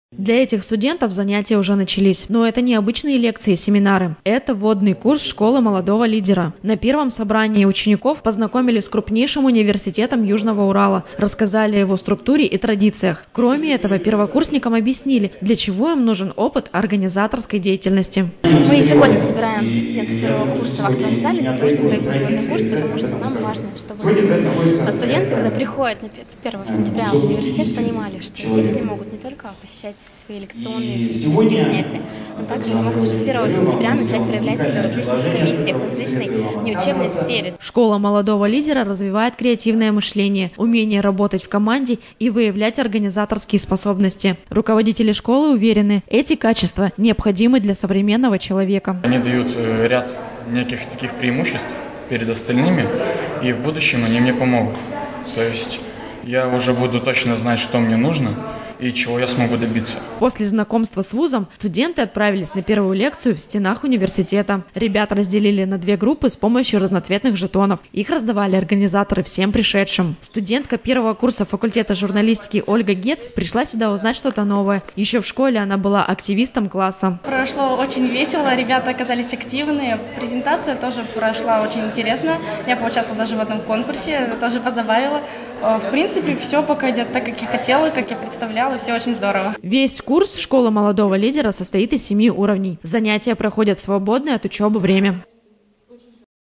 Курсы «Школы Молодого Лидера»
В ЮУрГУ стартовала Школа молодого лидера. Ежегодно слушателями уникального проекта становятся сотни первокурсников.
Для этих студентов занятия уже начались. Но это не обычные лекции и семинары. Это вводный курс «Школы Молодого Лидера». На первом собрании участников познакомили с крупнейшим университетом Южного Урала, рассказали о его структуре и традициях. Кроме этого первокурсникам объяснили, для чего им нужен опыт организаторской деятельности.
Школа молодого лидера развивает креативное мышление, умение работать в команде и выявляет организаторские способности. Руководители школы  уверены, эти качества необходимы для современного человека,
После знакомства с вузом студенты отправились на первую лекцию в стенах университета. Ребят разделили на две группы с помощью разноцветных жетонов. Их раздавали организаторы всем пришедшим.
Весь курс школы молодого лидера состоит из семи уровней. Занятия проходят в свободное от учебы время.